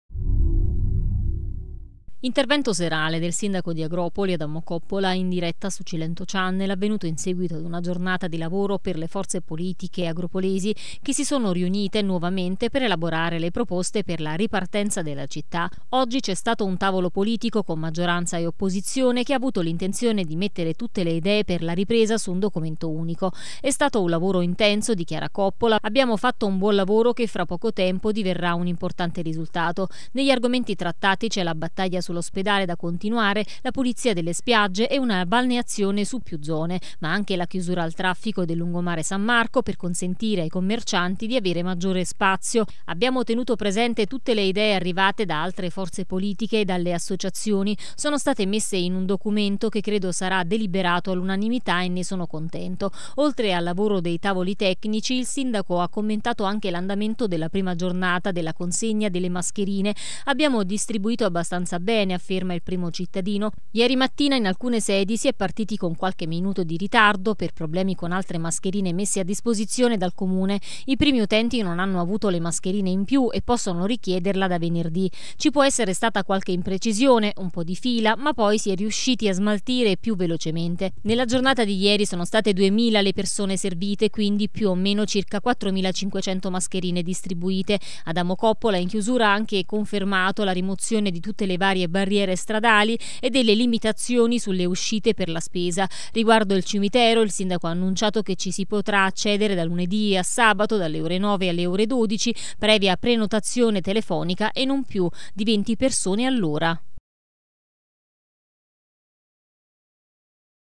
Agropoli, tavoli tecnici, Coppola: " Ieri lavoro intenso con maggioranza e opposizione”
Intervento serale del Sindaco di Agropoli Adamo Coppola, in diretta su Cilento Channel, avvenuto in seguito ad una giornata di lavoro per le forze politiche agropolesi, che si sono riunite nuovamente per elaborare le proposte per la ripartenza della città.